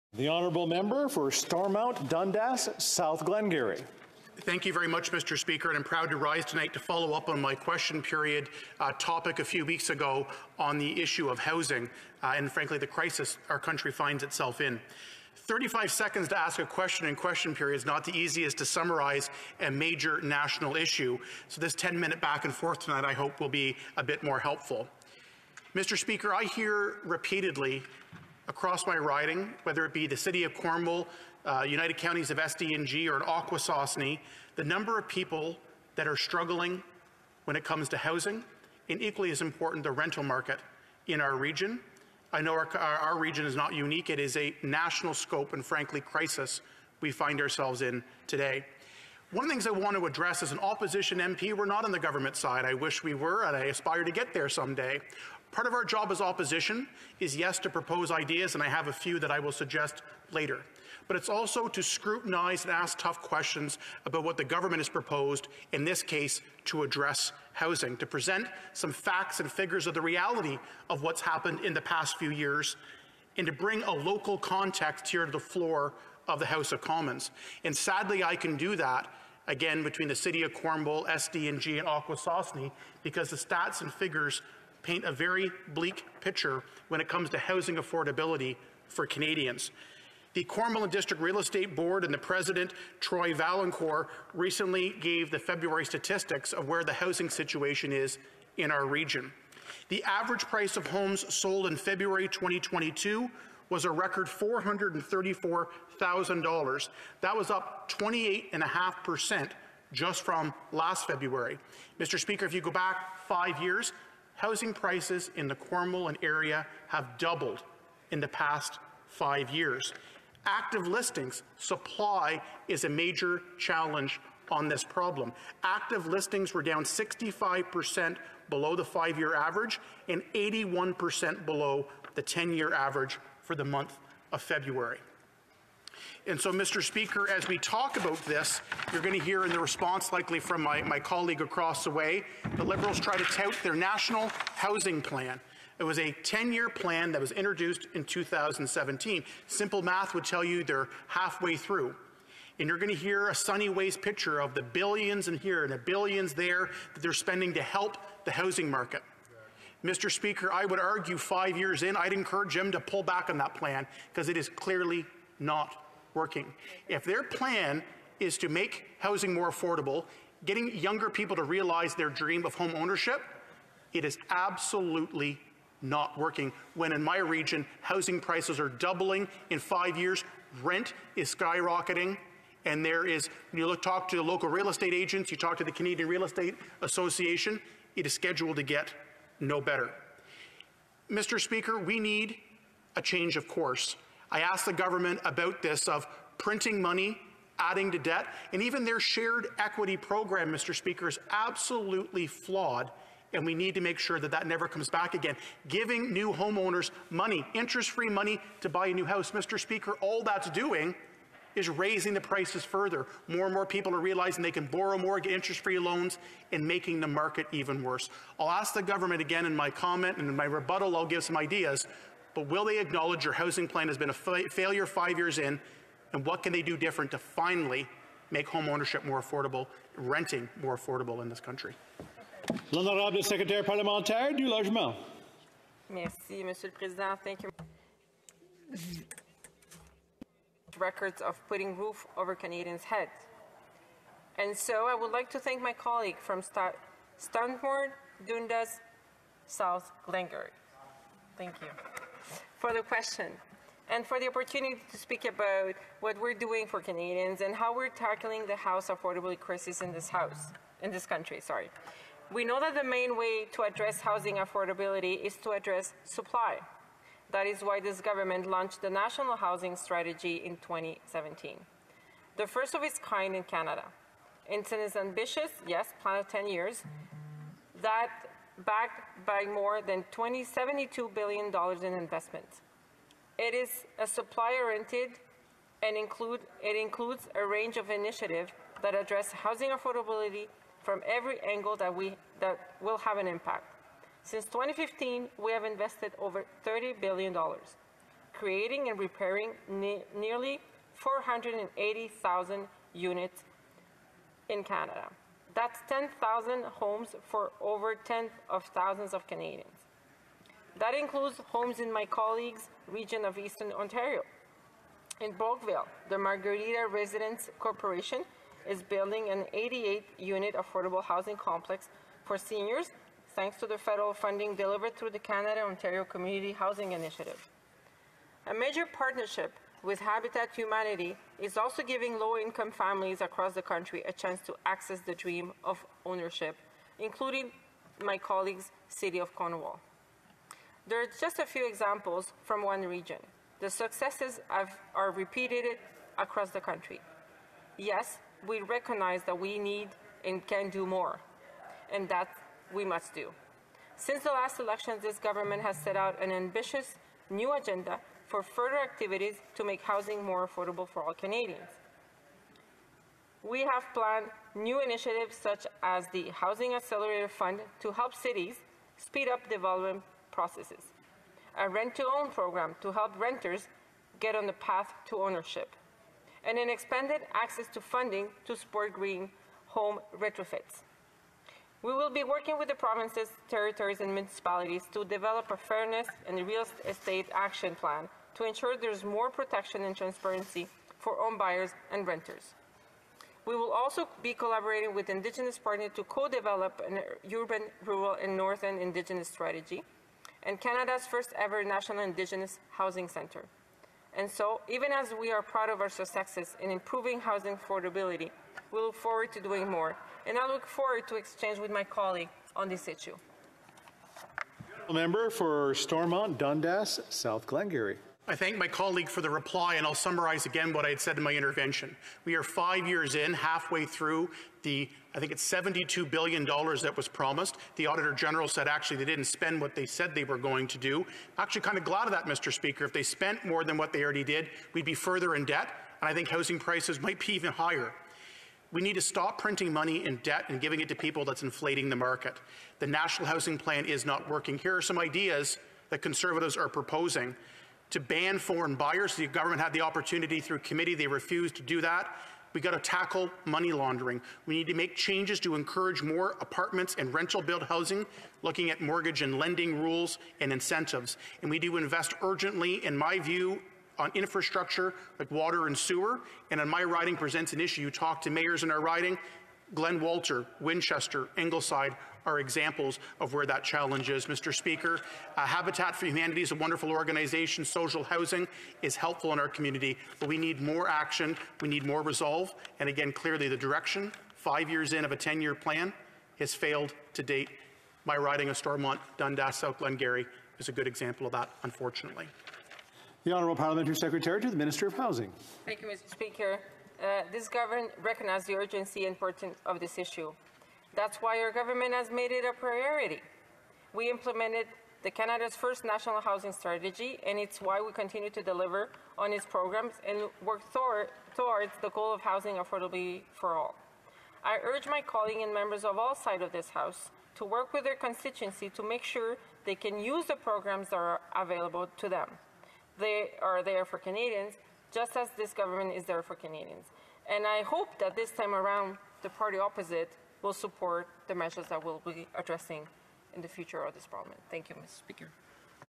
The Liberals' Housing Plan Has Doubled Prices in Cornwall & Area
We need a change in approach to tackling housing and rental affordability in Canada.

In 2017, the Liberals created their National Housing Strategy to "change the face of housing in Canada forever." Well, their efforts have changed the housing market- but in the wrong way. In those five years, housing prices have doubled in our region, and are expected to get even worse.

Listen below on my call for a new approach, including some ideas of how we can get more supply, and get prices down in Canada.